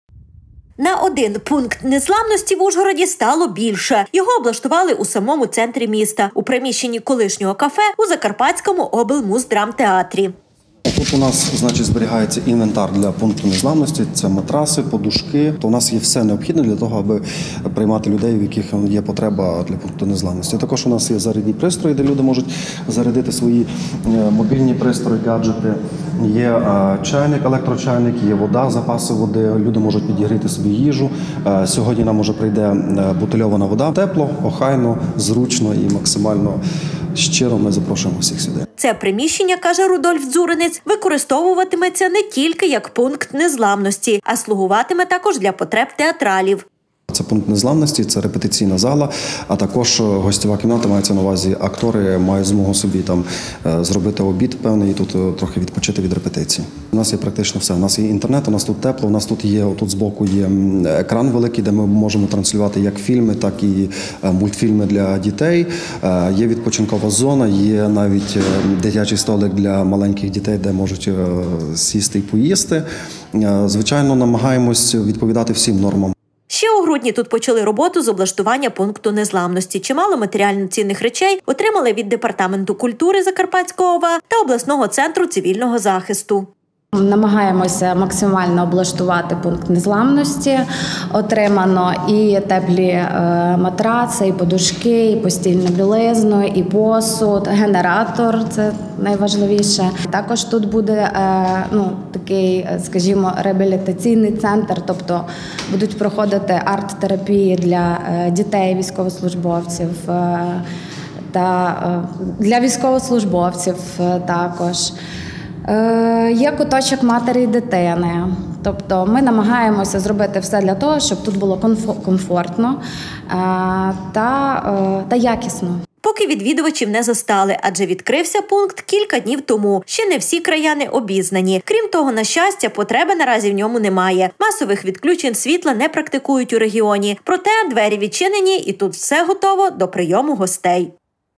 Пункт незламності у театрі
На один пункт незламності в Ужгороді стало більше! Його облаштували у самому серці міста - у приміщенні колишнього кафе у Закарпатському облмуздрамтеатрі. Це приміщення, каже, Рудольф Дзуринець, використовуватиметься не тільки, як пункт незламності.  А слугуватиме також для потреб театралів. Ще у грудні  тут почали роботу з облаштування пункту незламності. Чимало матеріально-цінних речей отримали від департаменту культури Закарпатської ОВА та обласного Центру цивільного захисту. Поки відвідувачів не застали, адже відкрився пункт кілька днів тому, ще не всі краяни обізнані. Крім того, на щастя, потреби наразі у ньому не має, масових відключень світла не практикують у регіоні. Проте  двері відчинені і тут все готово до прийому гостей!